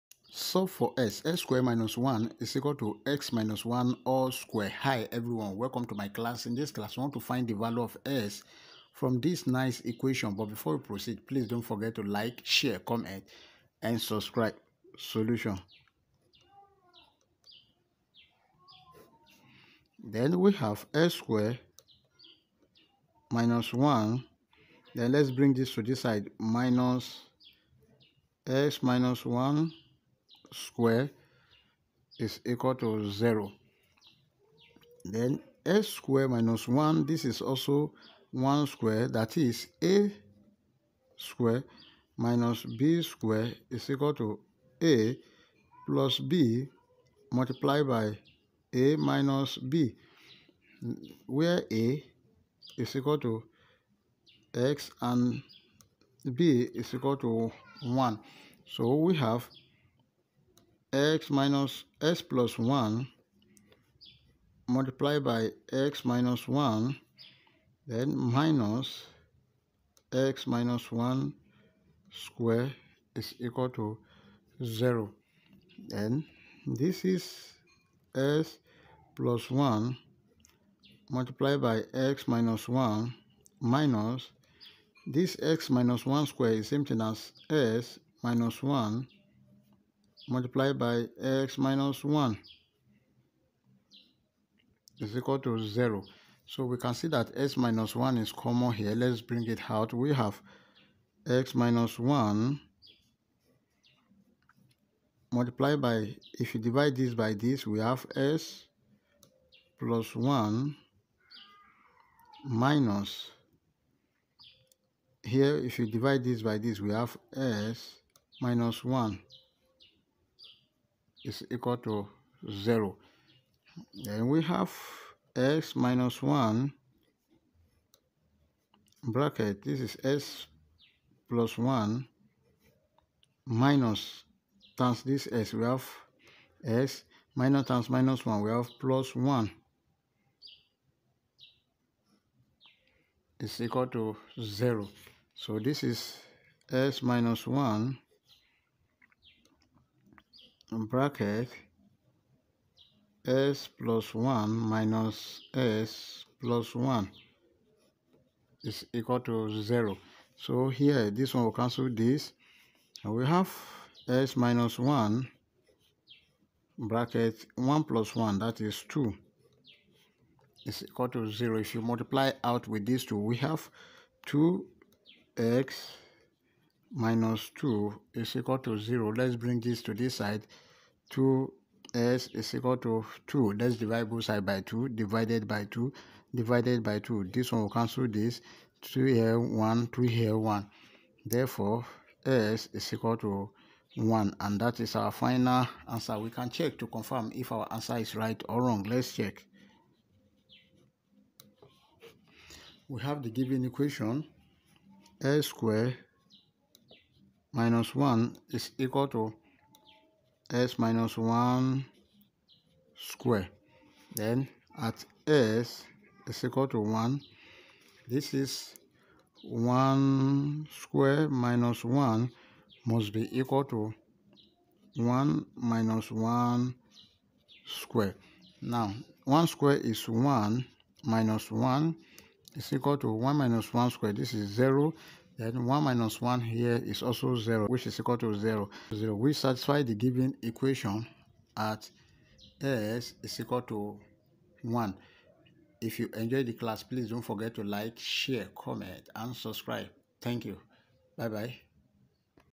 A Beautiful Math Problem X^2-1=(X-1)^2
How to Solve For X from This Nice Math  Problem,